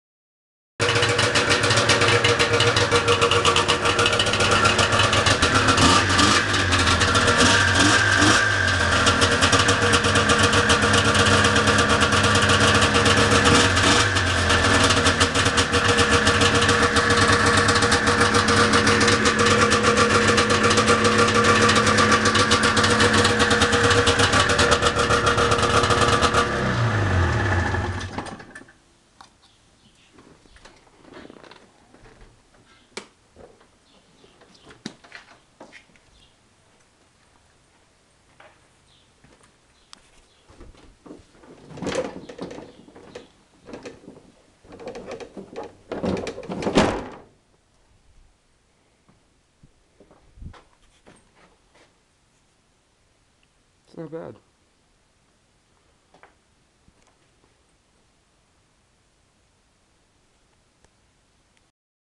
89 Arctic Cat El Tigre 440
im into this sled for only 20$ 1989 arctic cat 440 El Tigre. grr..
needs on off switch...and a track...but rides as is